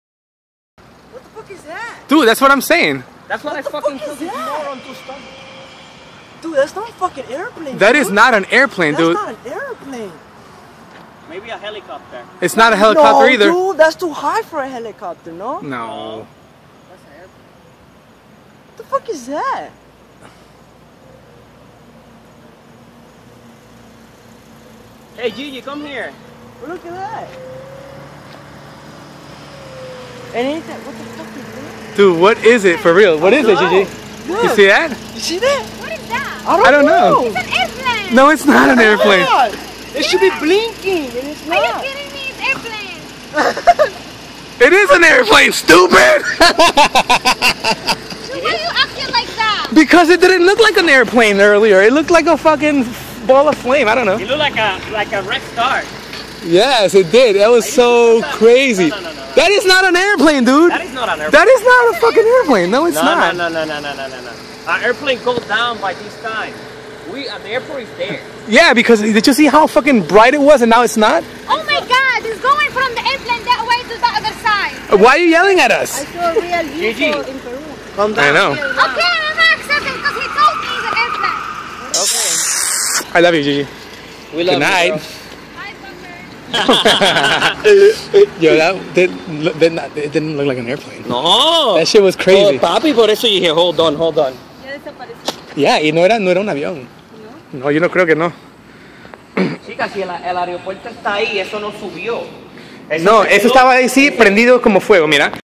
Copy of Light chilling in the sky above Tampa, Fl.
Right above international Plaza, walking to our cars after work, this light was spoted. You be the judge.